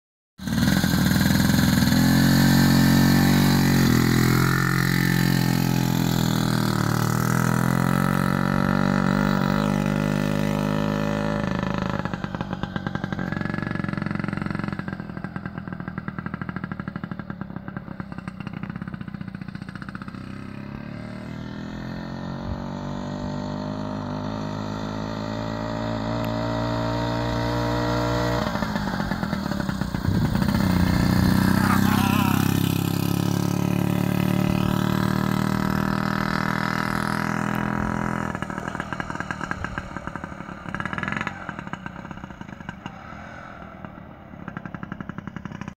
1934 Coventry Eagle first ride
Very first ride after a 4 year restoration. What a great day.